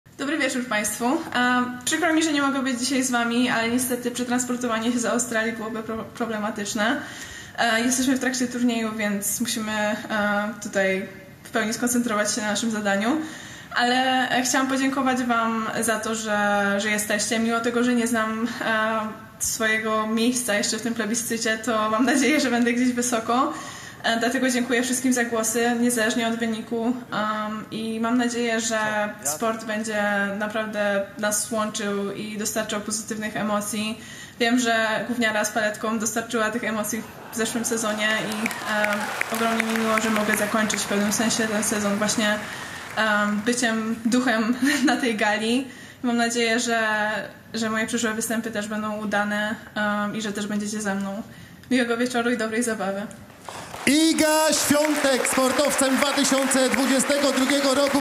Iga Świątek nawiązała do "g**niary z paletkom" w trakcie przemówienia na Gali Mistrzów Sportu
Mistrzyni! ;) I zdobywczyni tytułu Sportowca Roku 2022!